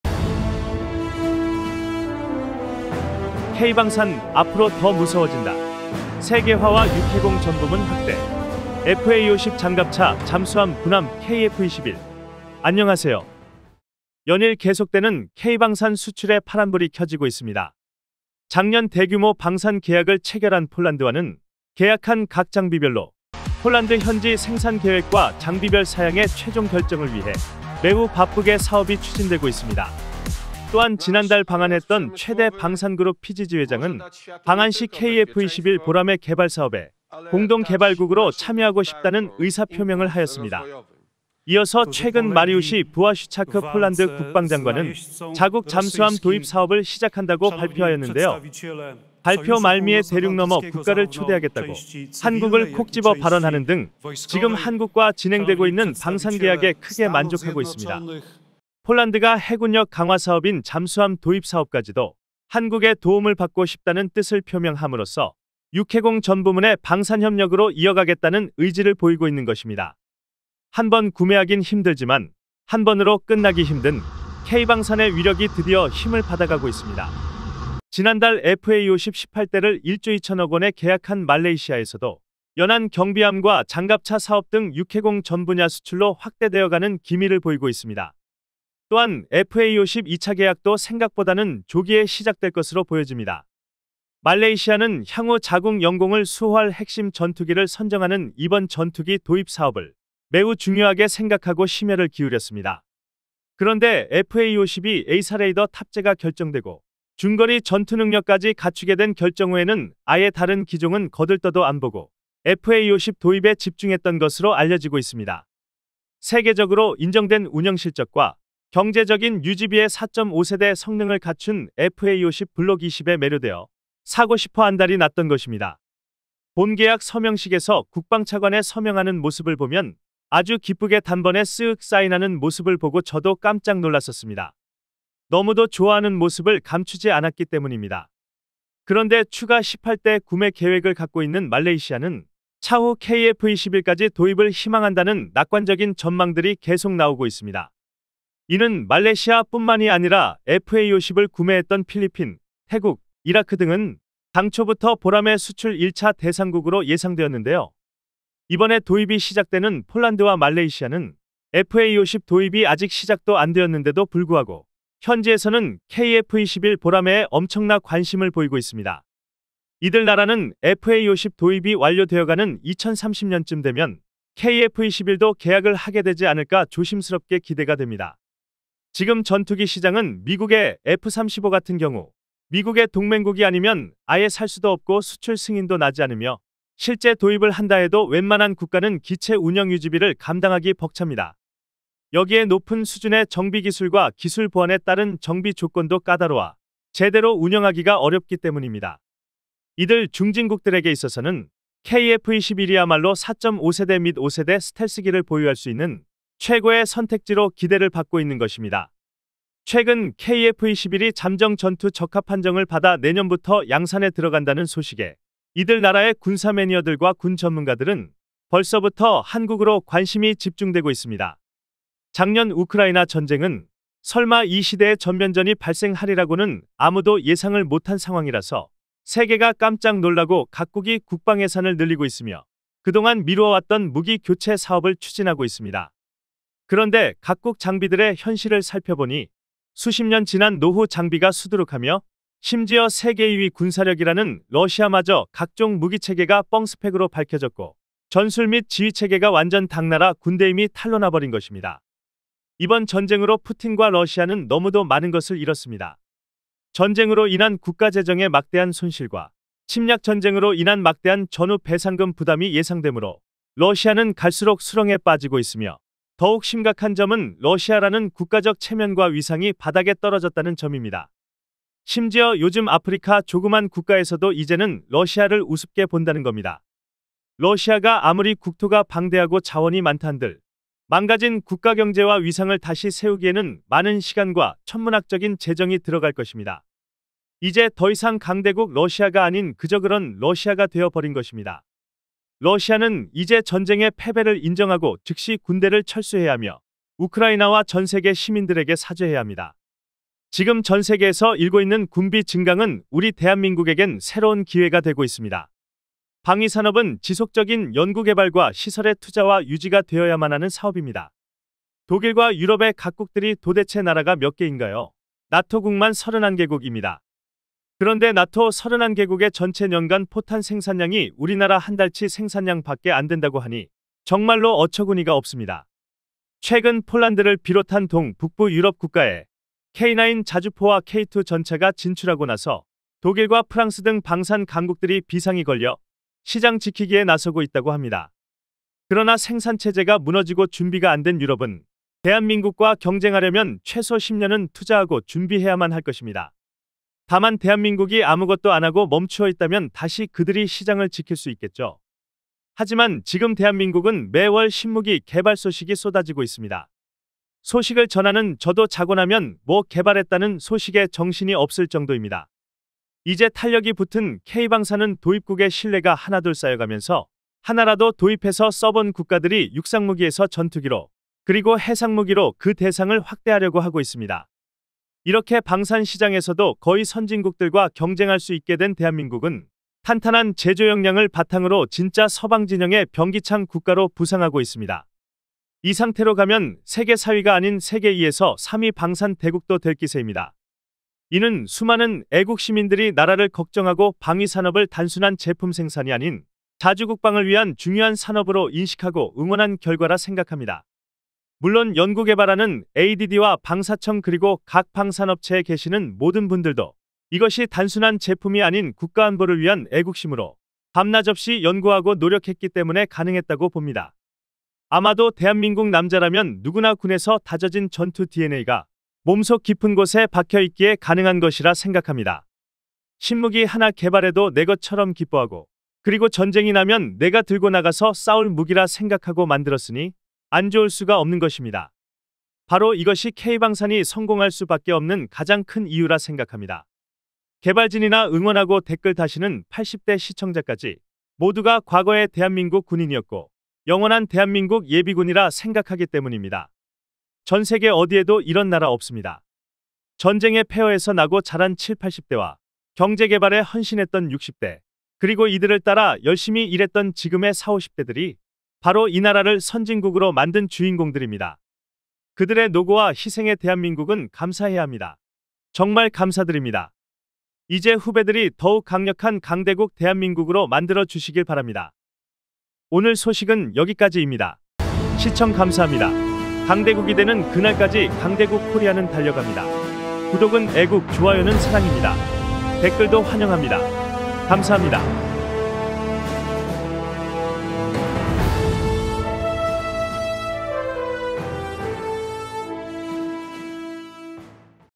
K방산.앞으로 더 무서워진다 . 육해공 전부문 확대. FA50.장갑차.잠수함.군함.KF21..
안녕하세요
연일 계속되는 K방산 수출에 파란 불이 켜지고 있습니다
작년 대규모 방산 계약을 체결한 폴란드는 계약한 각 장비별로 폴란드 현지 생산계획과 장비별 사양의 최종 결정을 위해 매우 바쁘게 사업이 추진되고 있습니다.
또한 지난달 방한 했던 최대 방산그룹 PGZ회장은 방한시 KF21 보라매 개발사업에 공동 개발국으로 참여 하고 싶다는 의사 표명을 하였습니다.
이어서 최근에는 폴란드 현지에서 국방장관이 폴란드 언론에 나와 자국 잠수함 도입 사업을 시작한다고 발표하였는데 발표 말미에 대륙넘어 국가를 초대하겠다고 한국을 콕찝어 발언 하는 등 지금 한국과 진행되고 있는 방산계약에 크게 만족하고 있습니다.
폴란드가 해군력 강화사업인 잠수함 도입사업까지도 한국의 도움을 받고 싶다는 의사를 표명 함으로써 육해공 전부문의 방산협력으로 이어 가겠다는 의사를 보이고 있습니다.
한번 구매하긴 힘들지만 한번으로 끝나기 힘든 K방산의 위력이 드디어 힘을 받아가고 있습니다.

또한 지난달 FA50 18대를 1조2천억원에 계약한 말레이시아에도 육해공 전 분야로 수출로 확대되어 가는 기미가 보이고 있습니다